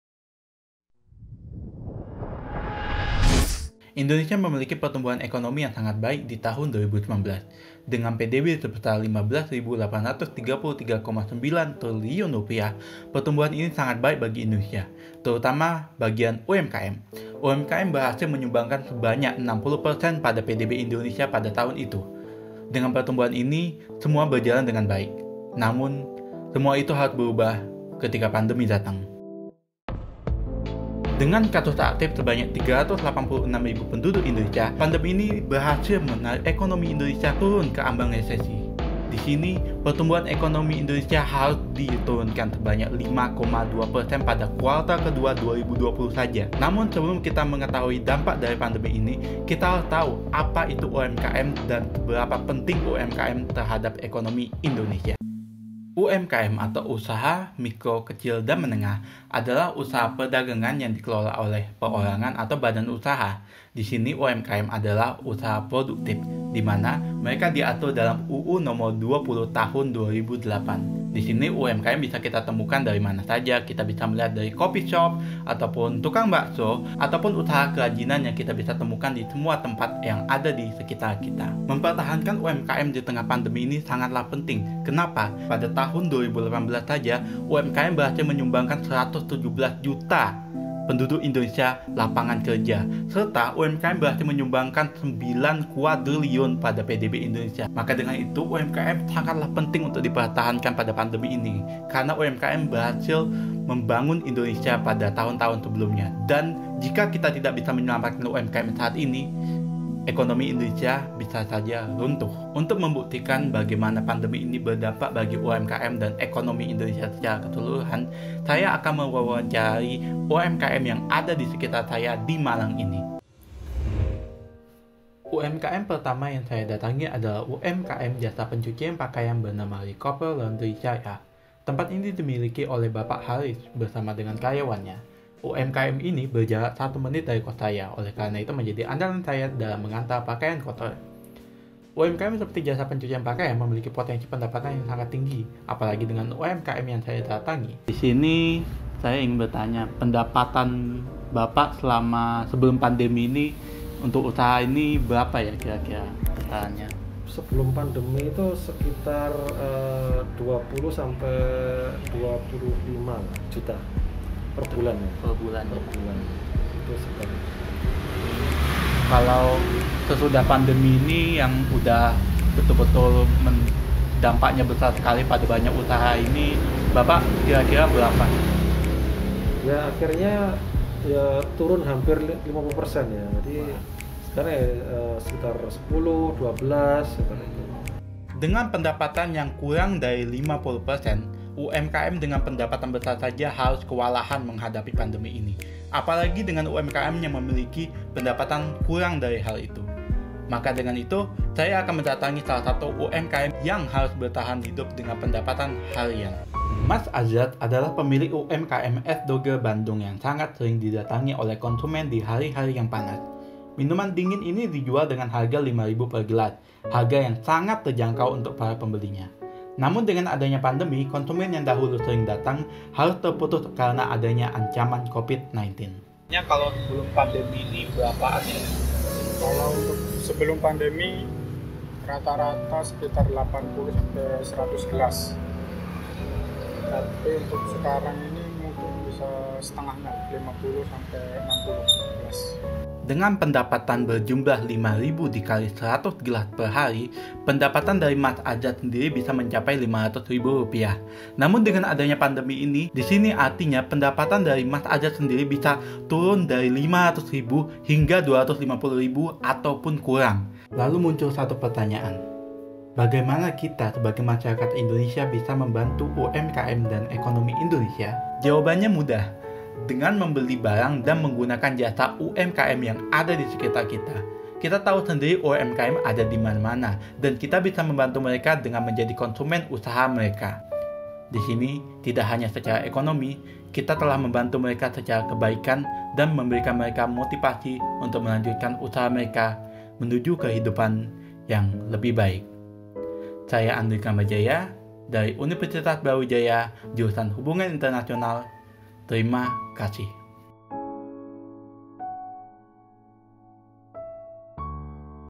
UMKM, Pandemi, dan Kita - Universitas Brawijaya for PNMHII XXXII
Universitas Brawijaya delegate presents an intriguing video to participate in the PNMHII XXXII Chamber Video Competition which will be held on 3-7th of November 2020 in Universitas Muhammadiyah Yogyakarta.

Make sure to support them by liking this video!